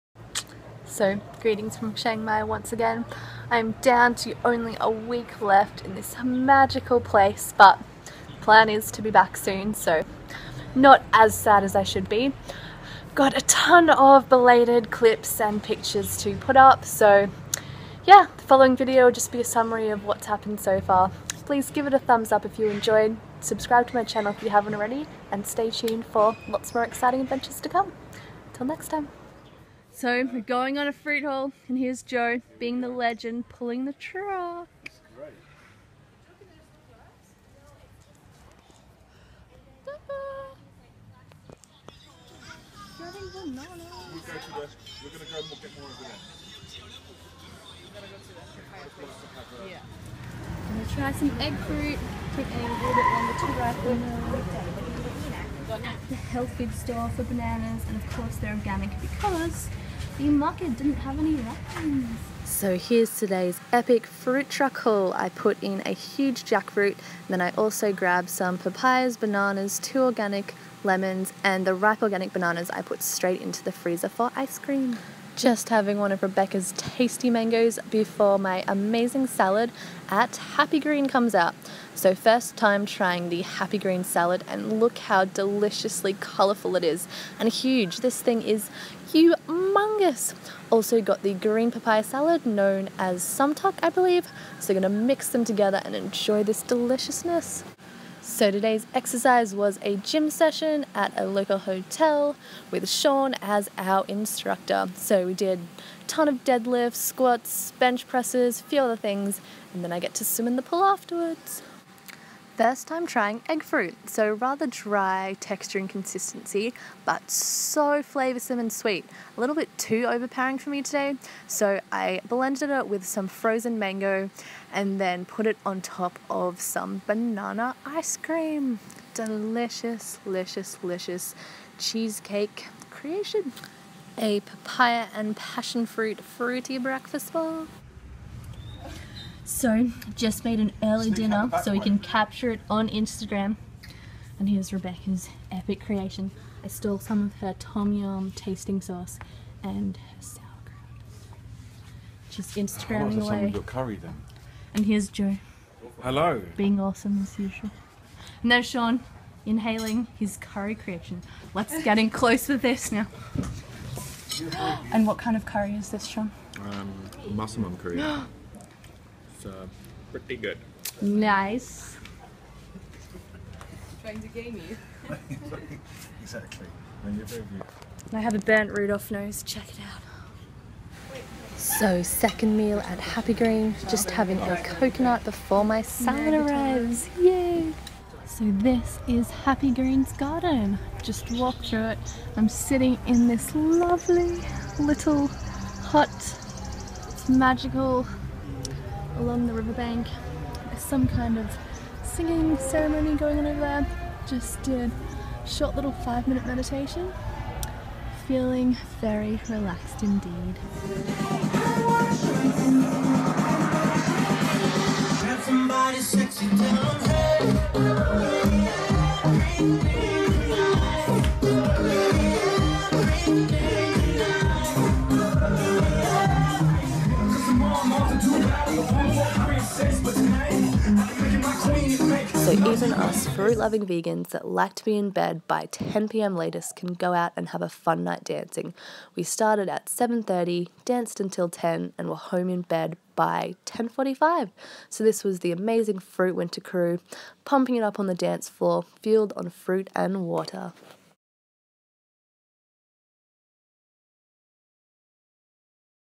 Raw/801010 Vegan food & dance party Vlog - Thailand FWF Jan 2015
My Chiang Mai Thailand & belated Fruit Winter Festival 2015 adventures continue!

PEACE, KALE, BANANAS & LIVE VEGAN :) Xx